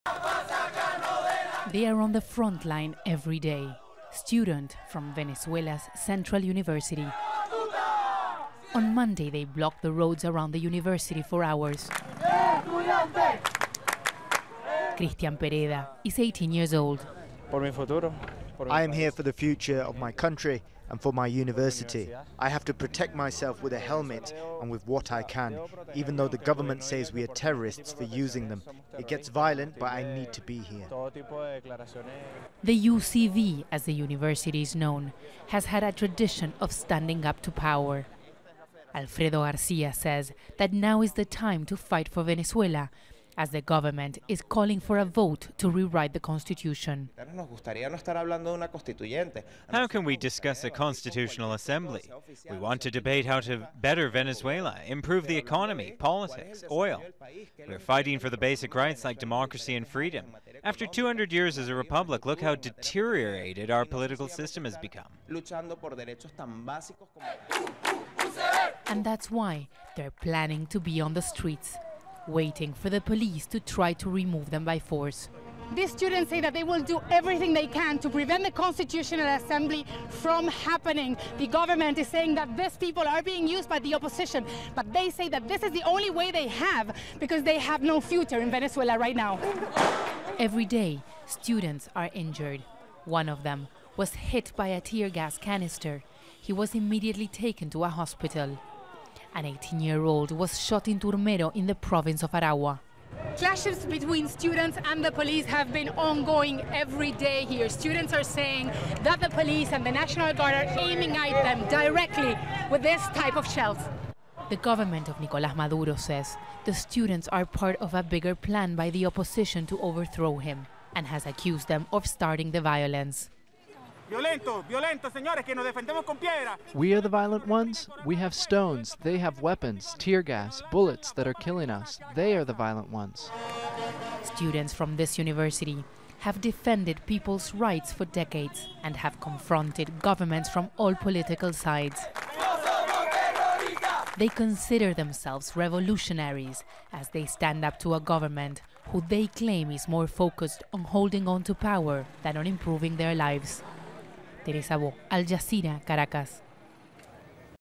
Venezuela students protest against bid to rewrite constitution
Students across Venezuela are protesting against President Nicolas Maduro's planned constituent assembly, which could lead to a rewrite of the country's constitution and the government's consolidation of power.

The country is facing a prolonged economic and political crisis, with daily protests in the capital city calling for Maduro to step down from office and call fresh elections.

Al Jazeera's Teresa Bo reports from Caracas.